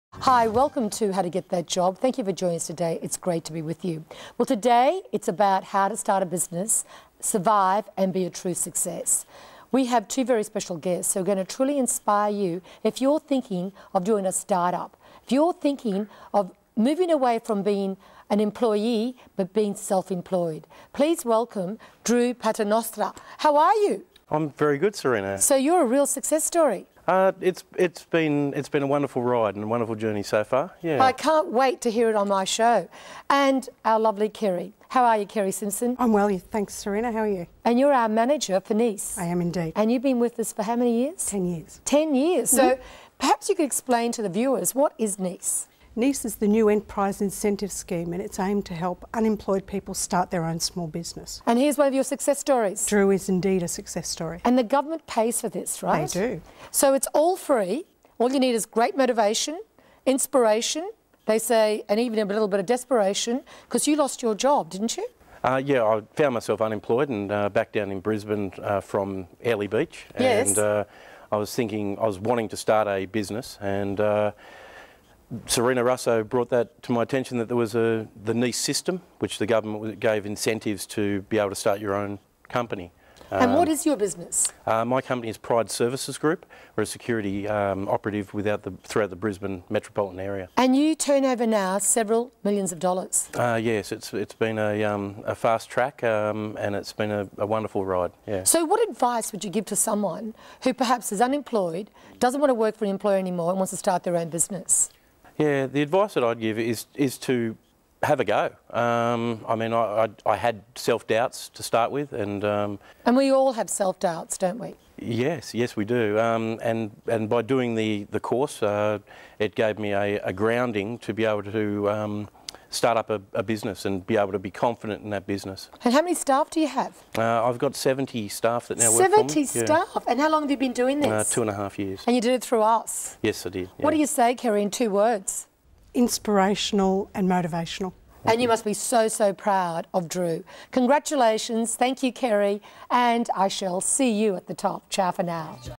Sarina Russo Job Access - Building a Million Dollar Business
Drew Paternoster, Director, Pride Services Group explains to Sarina Russo how he created a million dollar business after completing the New Enterprise Incentive Scheme programme at Sarina Russo Job Access.

National NEIS Program Manager Kerry Simpson said the Pride Services Group organises security guards and security patrols for businesses employing about 60 staff across 250 sites with a turnover of $2,000,000 per annum.